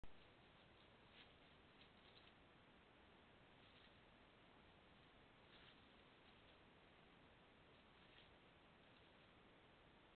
Buck Pooping
Buck pooping in the woods.